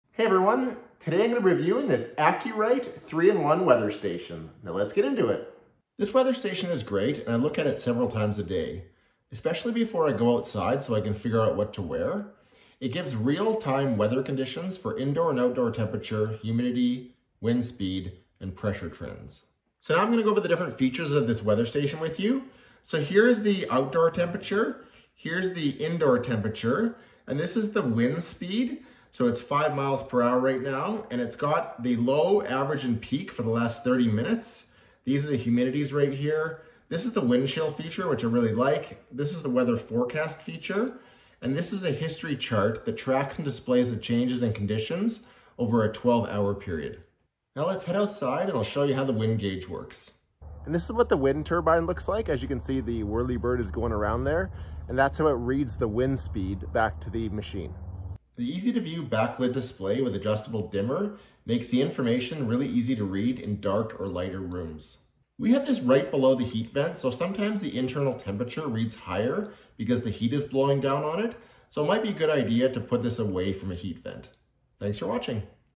AcuRite Notos (3-in-1) Weather Station Review and Demonstration
Check this weather station out

If you're looking for a reliable and accurate way to track the weather, the AcuRite Notos (3-in-1) Weather Station is the perfect solution. This station provides real-time readings for indoor and outdoor temperature, humidity, and wind speed, as well as pressure trends. Its advanced forecasting technology provides a hyperlocal, personalized weather forecast 12 hours from your current conditions. The illuminated LCD color display is easy to view and has an adjustable dimmer, making it perfect for any location. The all-in-one weather station also includes historical data for pressure, temperature, humidity, heat index, dew point, and wind chill. Stay one step ahead of the weather.